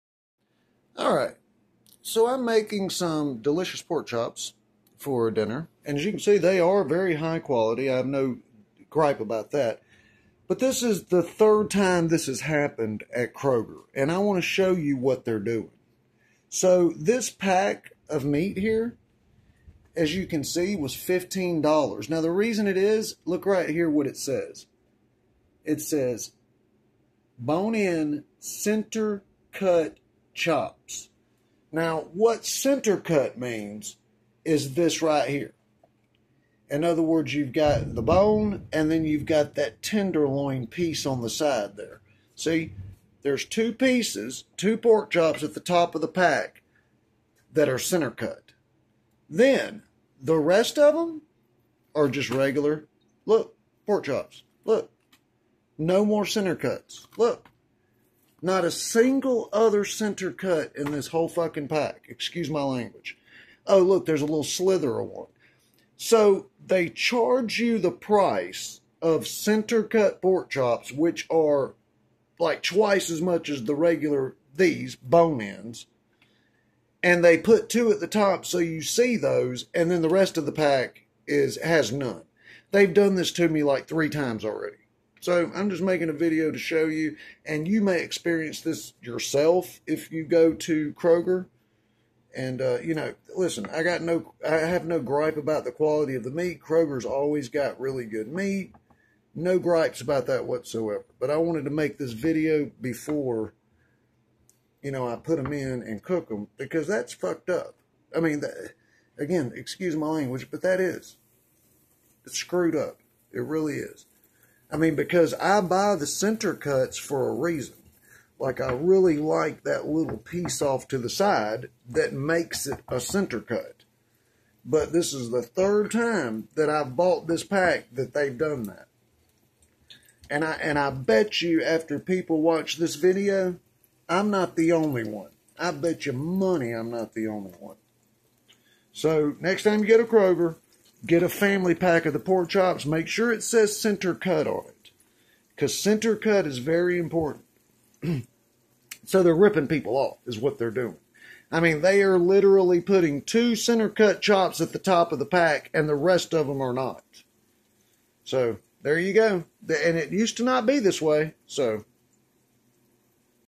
Kroger false advertising center cut chops
This is the 3rd time I have bought a family pack of center cut chops from Kroger, only to find there's two at the top and the rest are regular non center cut chops. They are intentionally placing two center cuts at the top and hiding the regular bone in's underneath.